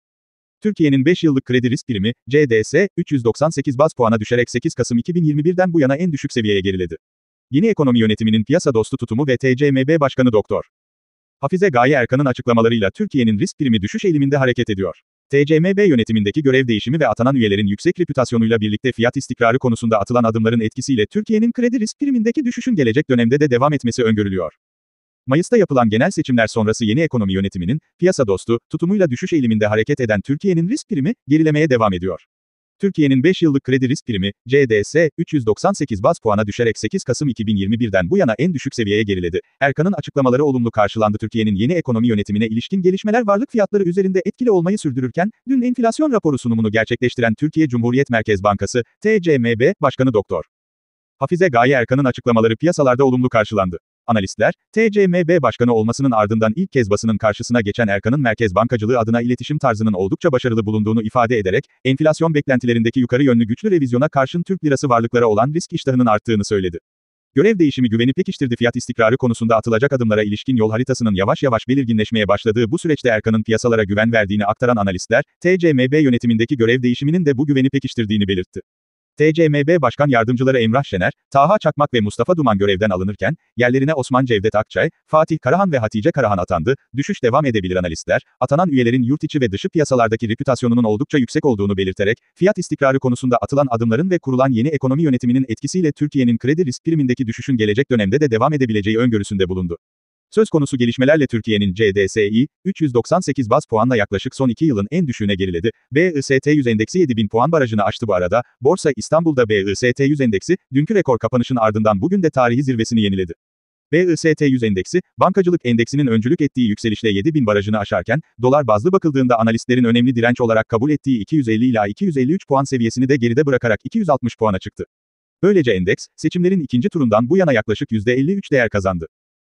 Türkiye'nin kredi risk primi düşüşe devam ediyor Türkiye'nin 5 yıllık kredi risk primi (CDS), 398 b
Türkiye'nin 5 yıllık kredi risk primi (CDS), 398 baz puana düşerek 8 Kasım 2021'den bu yana en düşük seviyeye geriledi. Yeni ekonomi yönetiminin piyasa dostu tutumu ve TCMB Başkanı Dr. Hafize Gaye Erkan'ın açıklamalarıyla Türkiye'nin risk primi düşüş eğiliminde hareket ediyor. TCMB yönetimindeki görev değişimi ve atanan üyelerin yüksek repütasyonuyla birlikte fiyat istikrarı konusunda atılan adımların etkisiyle Türkiye'nin kredi risk primindeki düşüşün gelecek dönemde de devam etmesi öngörülüyor
Mayısta yapılan genel seçimler sonrası yeni ekonomi yönetiminin "piyasa dostu" tutumuyla düşüş eğiliminde hareket eden Türkiye'nin risk primi, gerilemeye devam ediyor. Türkiye'nin 5 yıllık kredi risk primi (CDS), 398 baz puana düşerek 8 Kasım 2021'den bu yana en düşük seviyeye geriledi.ERKAN'IN AÇIKLAMALARI OLUMLU KARŞILANDITürkiye'nin yeni ekonomi yönetimine ilişkin gelişmeler varlık fiyatları üzerinde etkili olmayı sürdürürken, dün Enflasyon Raporu sunumunu gerçekleştiren Türkiye Cumhuriyet Merkez Bankası (TCMB) Başkanı Dr. Hafize Gaye Erkan'ın açıklamaları piyasalarda olumlu karşılandı. Analistler, TCMB Başkanı olmasının ardından ilk kez basının karşısına geçen Erkan'ın merkez bankacılığı adına iletişim tarzının oldukça başarılı bulunduğunu ifade ederek, enflasyon beklentilerindeki yukarı yönlü güçlü revizyona karşın Türk lirası varlıklara olan risk iştahının arttığını söyledi.
GÖREV DEĞİŞİMİ GÜVENİ PEKİŞTİRDİFiyat istikrarı konusunda atılacak adımlara ilişkin yol haritasının yavaş yavaş belirginleşmeye başladığı bu süreçte Erkan'ın piyasalara güven verdiğini aktaran analistler, TCMB yönetimindeki görev değişiminin de bu güveni pekiştirdiğini belirtti. TCMB başkan yardımcıları Emrah Şener, Taha Çakmak ve Mustafa Duman görevden alınırken, yerlerine Osman Cevdet Akçay, Fatih Karahan ve Hatice Karahan atandı.DÜŞÜŞ DEVAM EDEBİLİRAnalistler, atanan üyelerin yurt içi ve dışı piyasalardaki repütasyonunun oldukça yüksek olduğunu belirterek, fiyat istikrarı konusunda atılan adımların ve kurulan yeni ekonomi yönetiminin etkisiyle Türkiye'nin kredi risk primindeki düşüşün gelecek dönemde de devam edebileceği öngörüsünde bulundu. Söz konusu gelişmelerle Türkiye'nin CDS'i, 398 baz puanla yaklaşık son 2 yılın en düşüğüne geriledi.BIST 100 ENDEKSİ 7 BİN PUAN BARAJINI AŞTIBu arada, Borsa İstanbul'da BIST 100 endeksi, dünkü rekor kapanışın ardından bugün de tarihi zirvesini yeniledi.
BIST 100 endeksi, bankacılık endeksinin öncülük ettiği yükselişle 7.000 barajını aşarken, dolar bazlı bakıldığında analistlerin önemli direnç olarak kabul ettiği 250-253 puan seviyesini de geride bırakarak 260 puana çıktı.
Böylece endeks, seçimlerin ikinci turundan bu yana yaklaşık yüzde 53 değer kazandı.